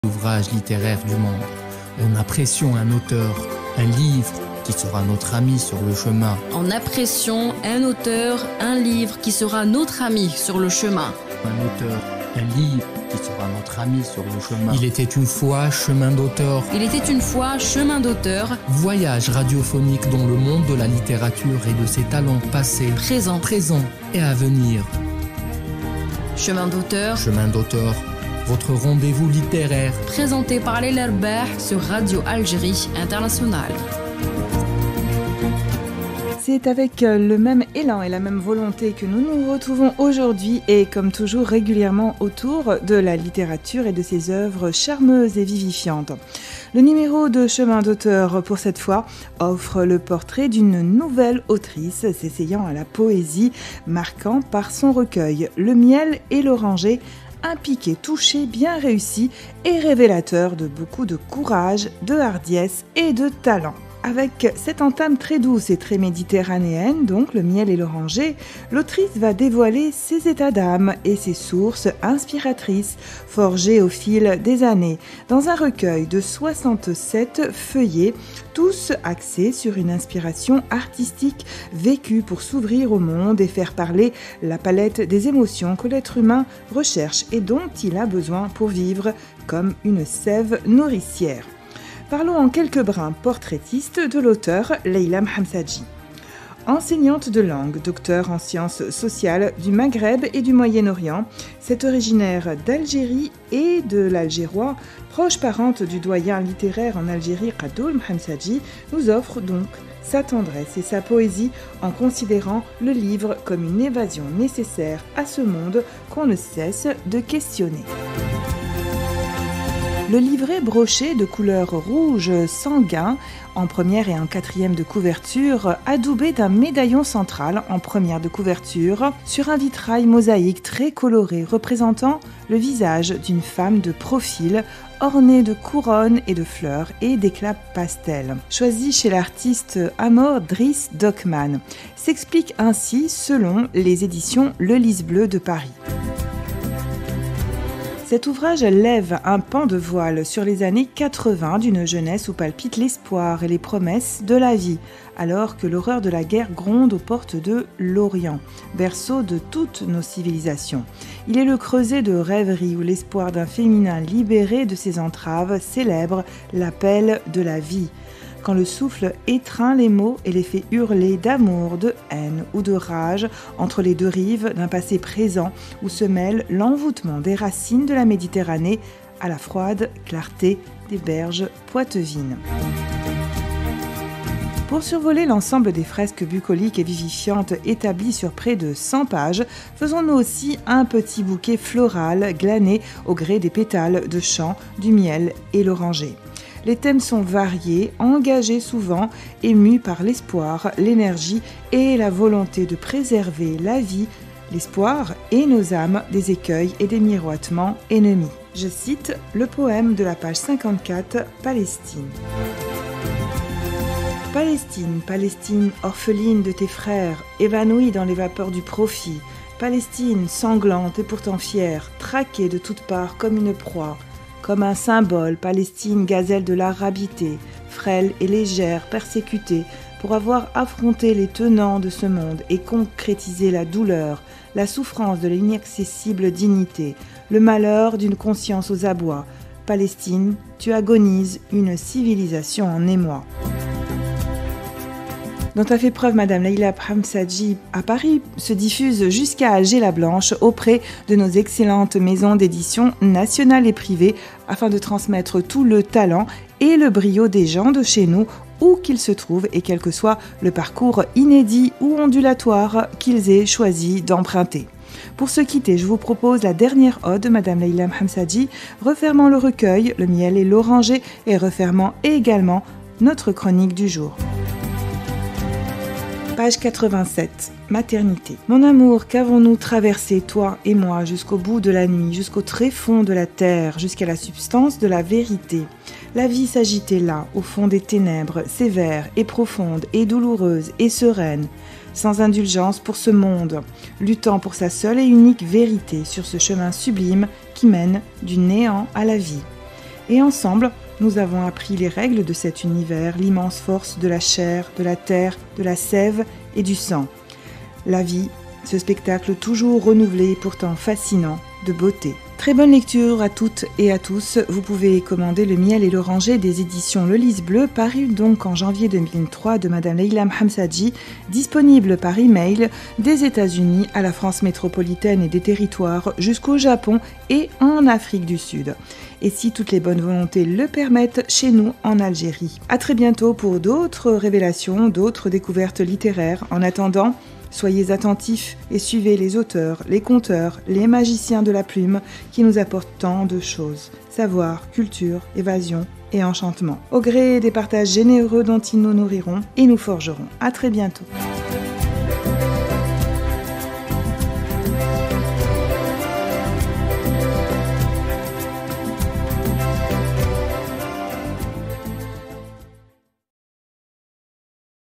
Chronique littéraire du livre Le miel et l'oranger" de Leila M'hamsadji
Si vous n'avez pas encore découvert "Le miel et l'oranger" de Leila M'hamsadji, vous devriez écouter la chronique de LEILA RABAH sur Radio Algérie Internationale.

Cet ouvrage lève un pan de voile sur les années 80 d’une jeunesse où palpitent l’espoir et les promesses de la vie, alors que l’horreur de la guerre gronde aux portes de l’Orient, berceau de toutes nos civilisations. Il est le creuset de rêveries où l’espoir d’un féminin libéré de ses entraves célèbre l’appel de la vie. Quand le souffle étreint les mots et les fait hurler d’amour, de haine ou de rage, entre les deux rives d’un passé-présent où se mêle l’envoûtement des racines de la méditerranée à la froide clarté des berges poitevines.